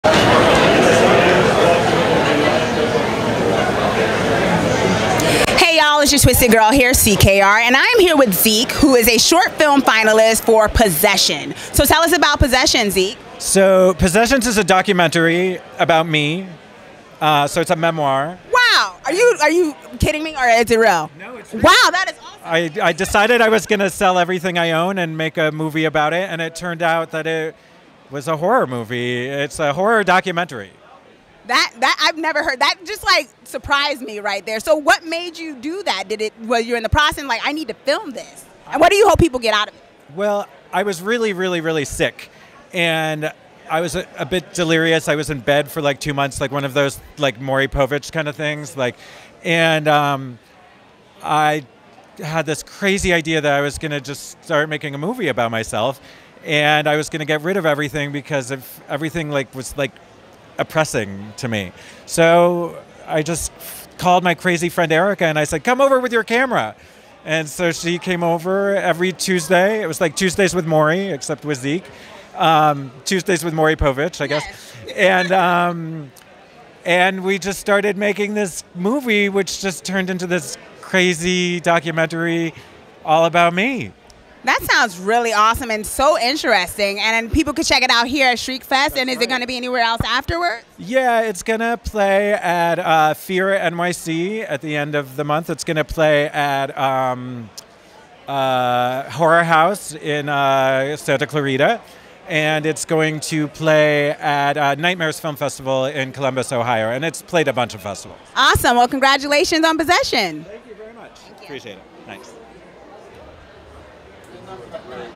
Shriekfest 2019 Red Carpet Interviews! Zeke Farrow!
Shriekfest 2019 Red Carpet Interviews! Chauncey K Robinson chats with  Zeke Farrow about his film "Possessions"!  Footage by Nick Danger!